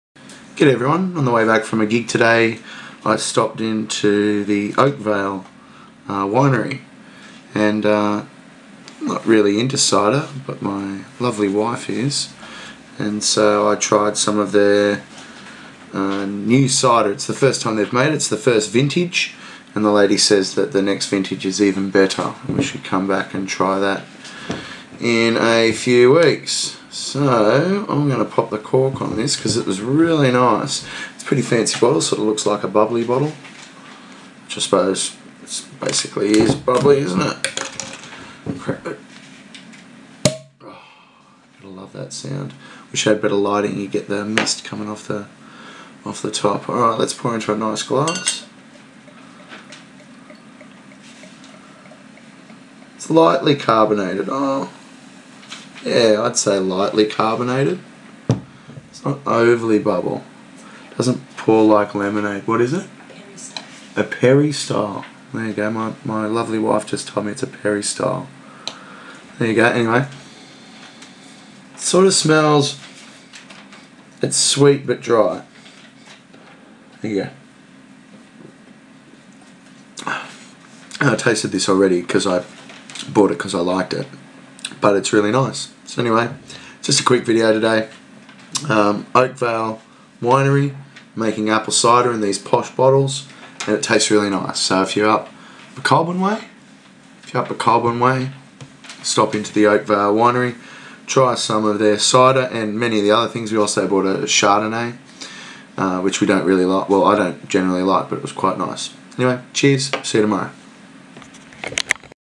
Oakvale Apple Cider 2012 Vintage Review
Took a drive back through wine country after a gig today and stopped in at Oakvale winery for a little taste. I picked up this bottle fancy apple cider for my wife, and thought I would pop the cork and have a taste for you guys.